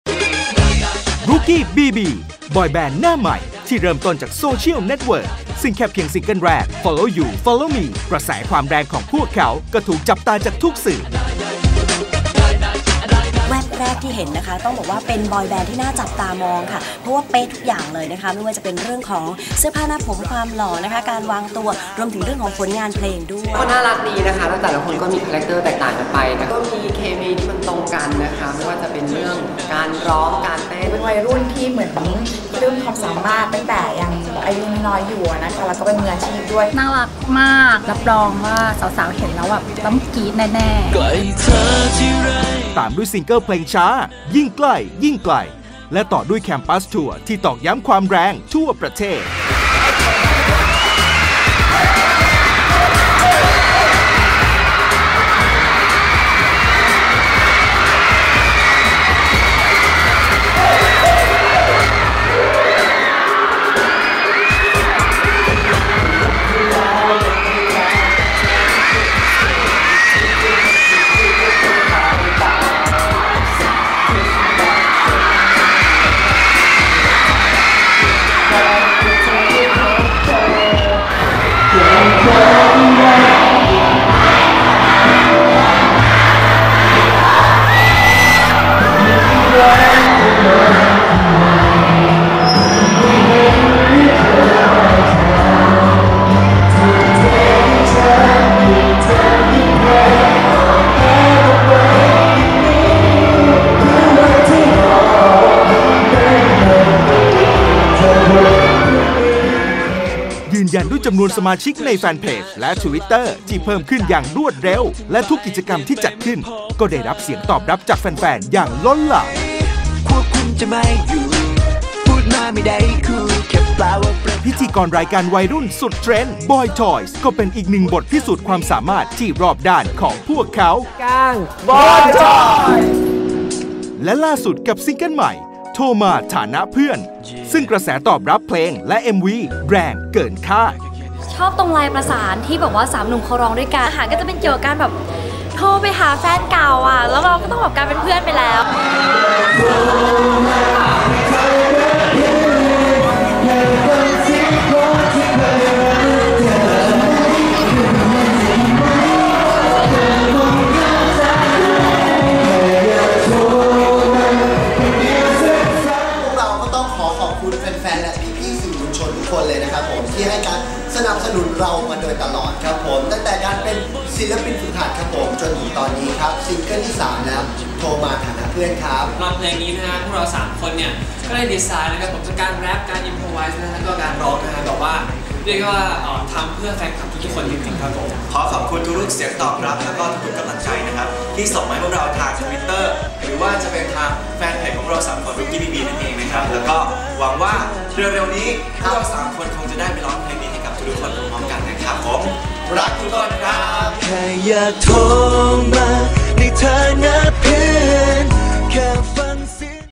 Scoop ความแรง Rookie BB
HOT ทั่วประเทศแล้ว กับกระแสความแรงอย่างต่อเนื่องของ 3 ศิลปินหนุ่ม Rookie BB ที่ตอนนี้ได้รับความนิยมและการตอบรับที่ดีจาก Fanclub ทั่วประเทศ รวมถึงเหล่า Caleb ที่เป็นดารา หรือแม้กระทั่งศิลปินด้วยกันเอง อยากรู้ว่า Rookie BB HOT ยังไง ไปรู้จักกับเพลงของพวกเขาได้ที่